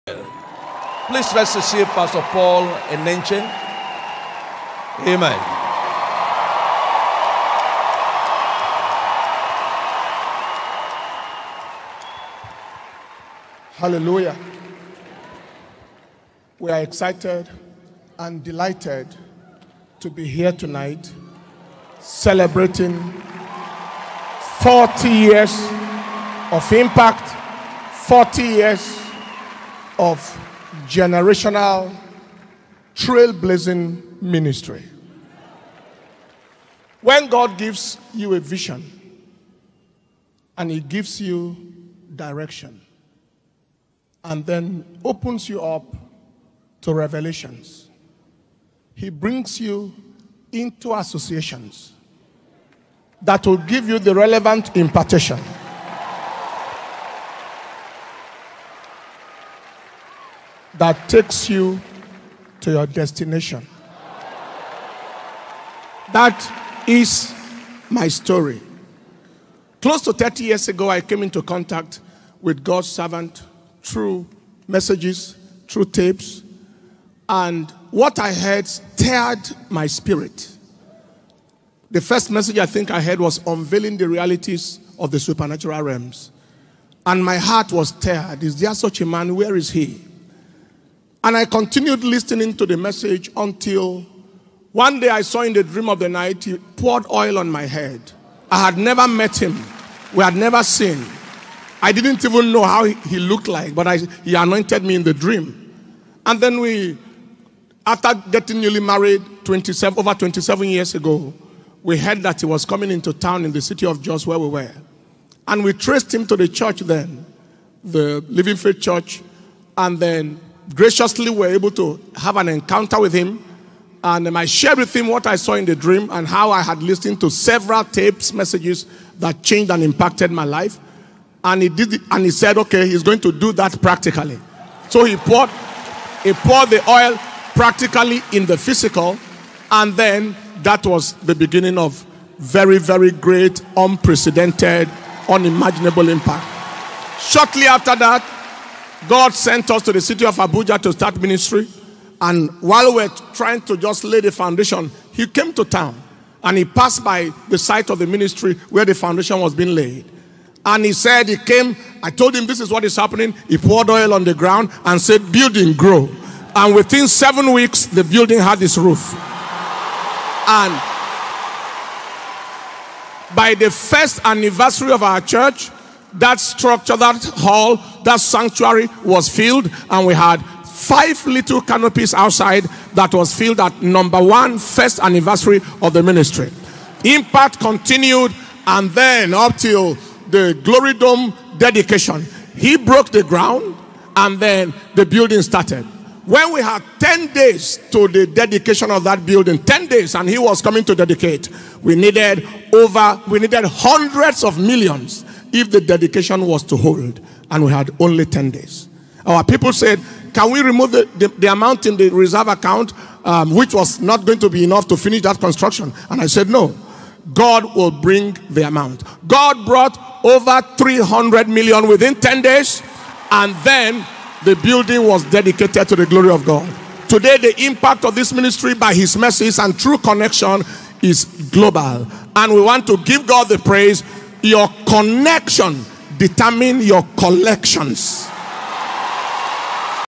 PASTOR PAUL ENENCHE: YOUR CONNECTION DETERMINES YOUR COLLECTION| BISHOP DAVID OYEDEPO'S IMPACT ON ME
Pastor Paul Enenche gives remarks on the impact of Bishop David Oyedepo on his ministry.  This is the 40th Anniversary Celebration of the Liberation Mandate of the Living Faith Church aka Winners Chapel. This prophetic feast has been tagged "Possessing your Promised Land” Deut. 11:8-28.
The liberation mandate is 40years.

To God alone be all the Glory

@winners chapel international Dartford
@living faith church worldwide
@official winners chapel maryland